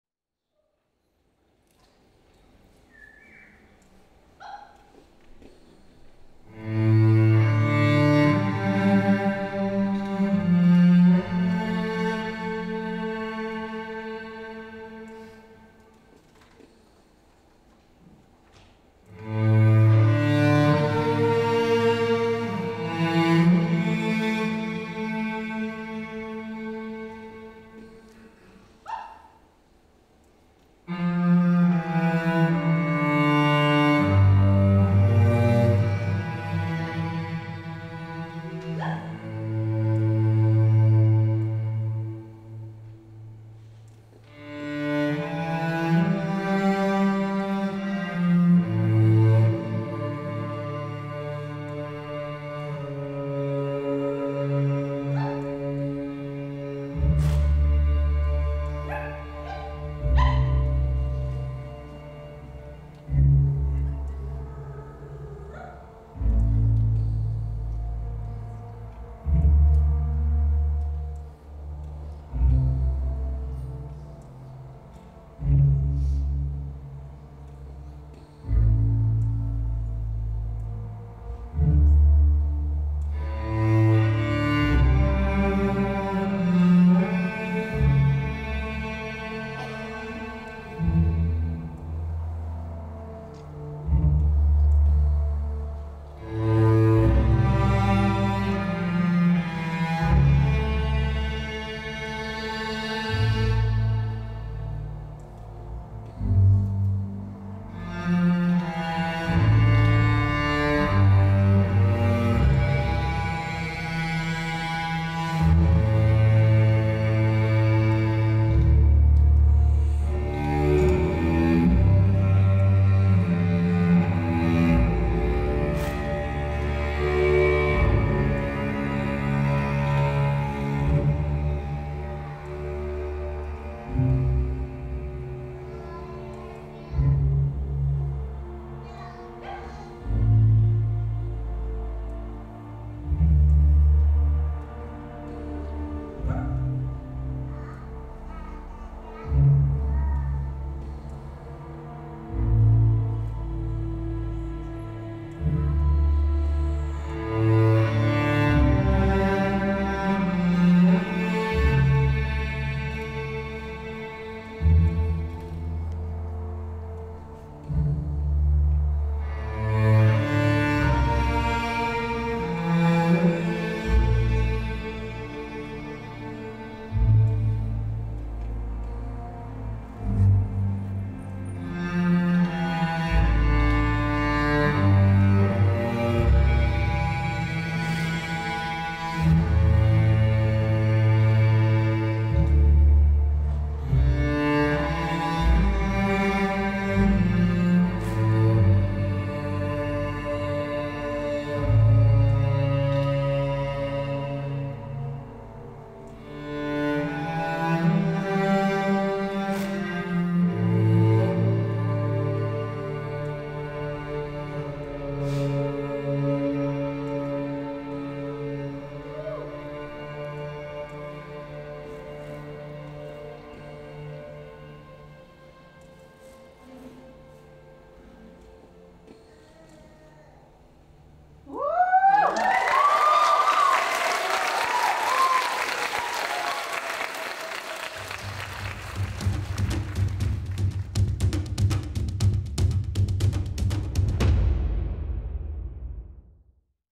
Jeana's flamenco fan solo at Bloomington Belly Dances 2023
Bloomington Belly Dances returns for its 20th anniversary show. I performed this improvised solo, using the flamenco fan dialect developed by the Siren Project in Warsaw, Poland.